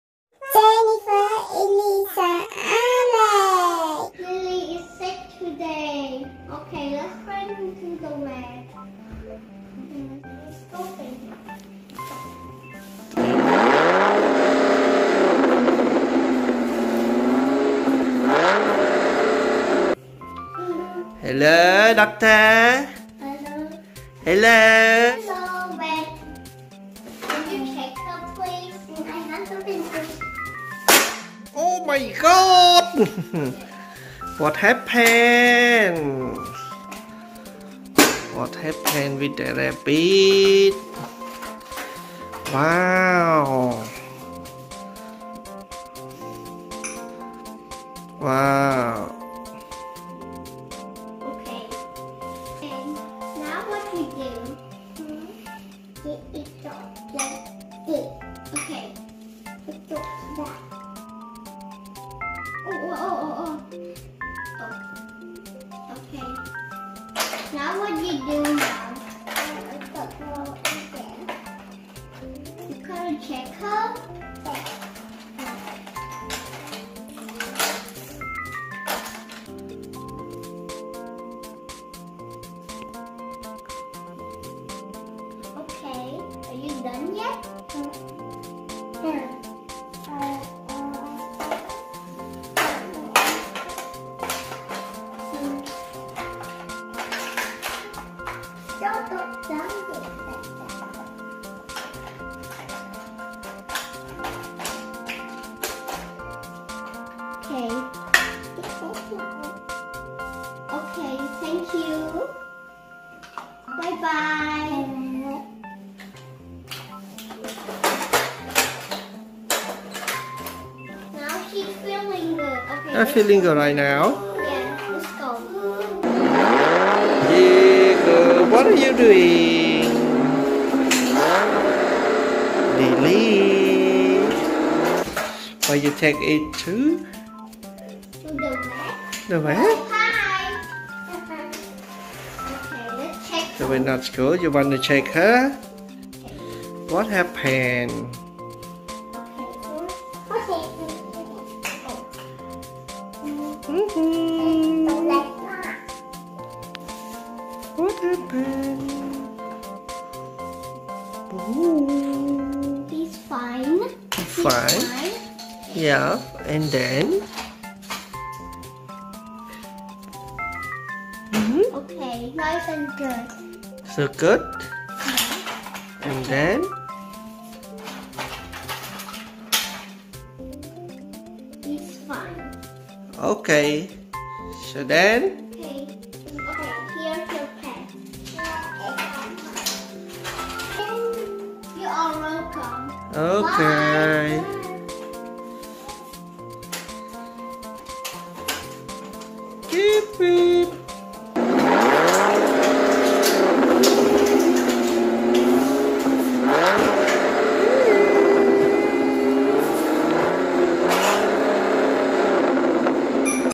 Jennifer And Elisa Pretend Play PET VET, Check Up for Rabbit Lilly
Jennifer And Elisa pretended play pet vet, gave rabbit lilly check up, Elisa pretended to be a vet and Jennifer brought the rabbit lilly checked up, they played and made some funs together. they all got a wonderful playtime with BUNNY lilly.